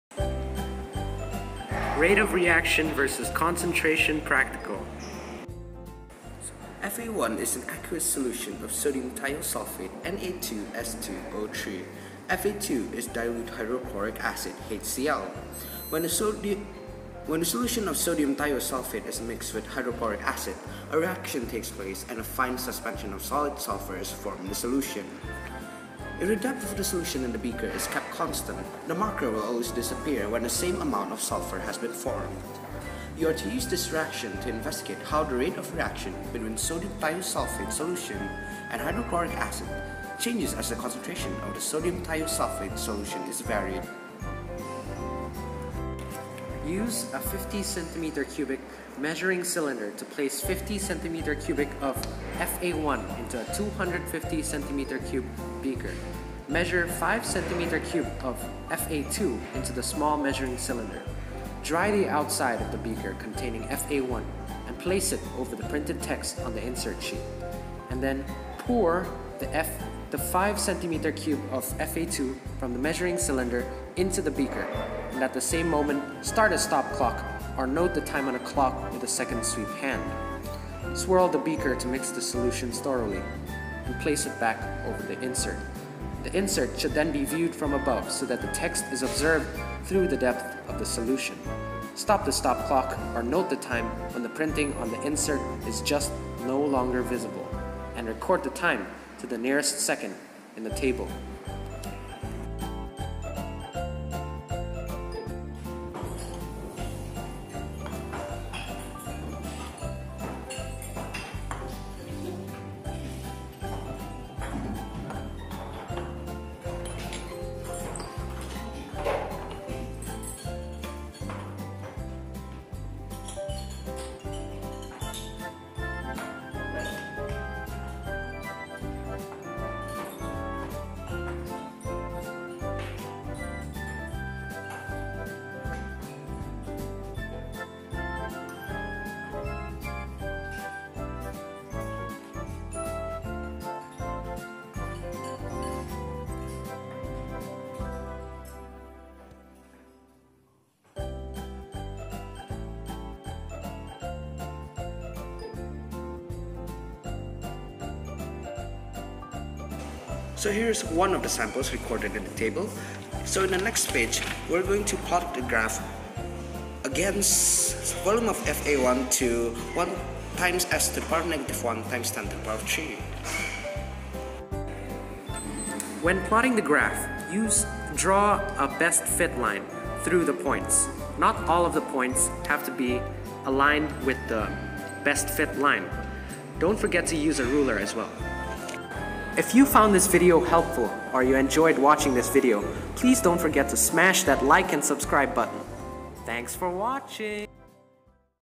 SODIUM THIOSULFATE and HYDROCHLORIC ACID REACTION TO MEASURE RATE AGAINST CONCENTRATION
Experiment to measure the rate with concentration of sodium thiosulfate.
Done with the voice of Rafael and Nico from JC1 ZEAL
BBSPIK CHEMISTRY
JAKARTA
GOPI K
2018